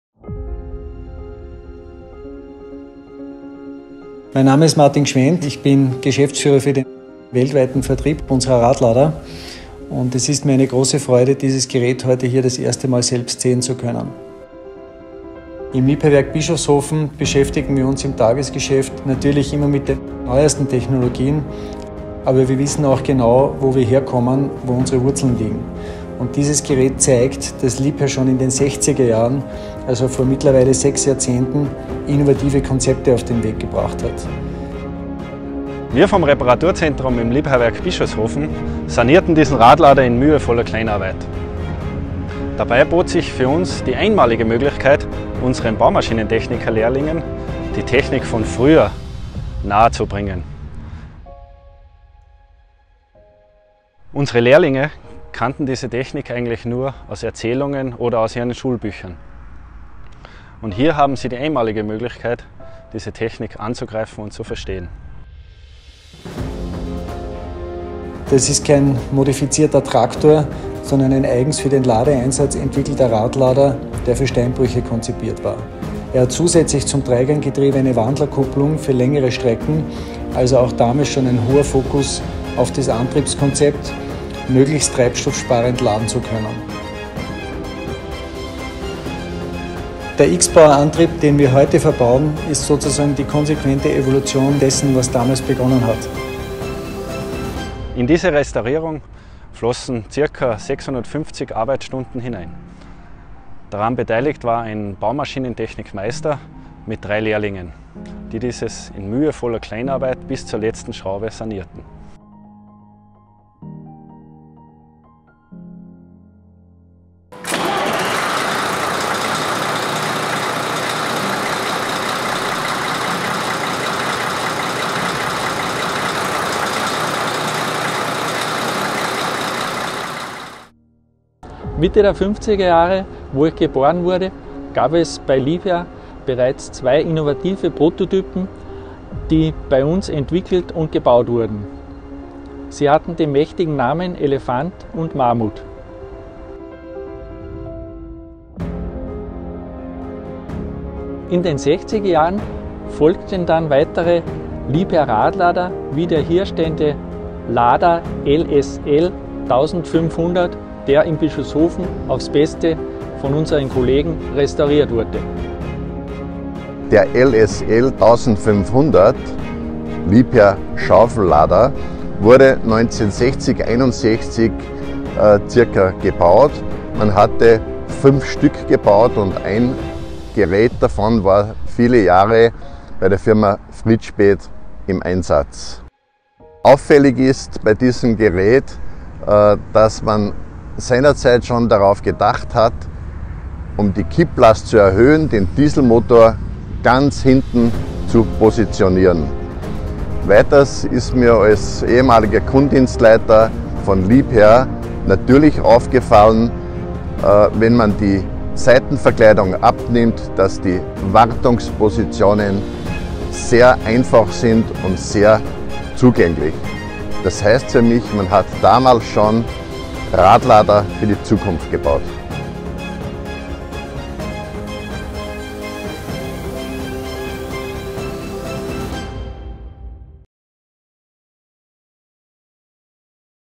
Liebherr - The LSL 1500 shovel loader
Over recent months, the Liebherr factory in Bischofshofen has dedicated itself to a project for real enthusiasts - the restoration of the oldest Liebherr wheel loader still in existence. A committed team of 11, around 650 working hours and plenty of passion were what it took to return the LSL 1500 shovel loader back to its former glory. This example from the 1960s is a wheel loader which was specifically designed for use in quarries. Now, a piece of Liebherr history can look forward to a happy future as an exhibit at the Bischofshofen wheel loader centre of excellence. Further information about the restoration can be found